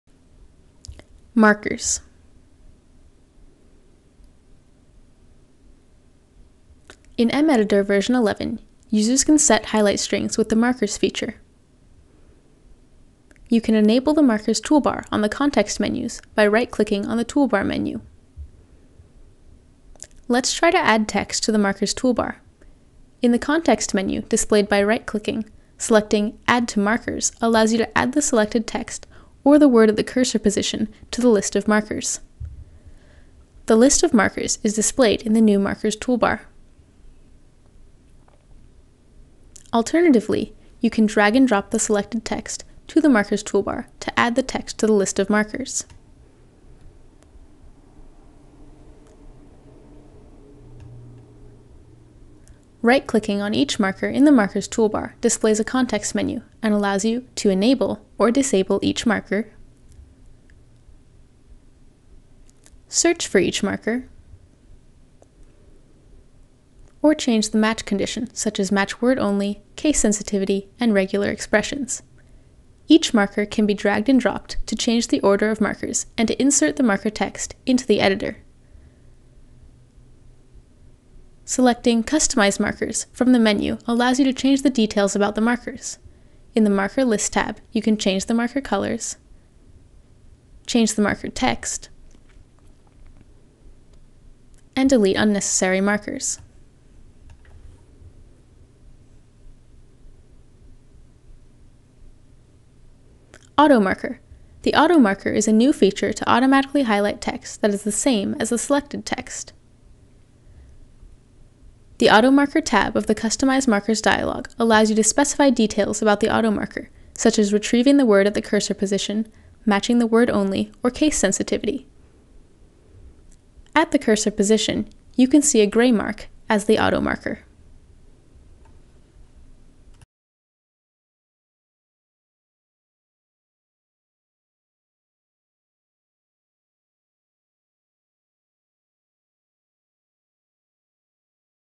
EmEditor v11 Feature - Markers
EmEditor currently allows users to set highlight strings. However, in previous versions, you could not add highlight strings temporarily. These limitations have been rectified in the new version.

The auto marker is a new feature to automatically highlight text that is the same as the selected text. The Auto Marker tab of the Customize Markers dialog allows you to specify details about the auto marker, such as retrieving the word at the cursor position, matching the word only, or case sensitivity.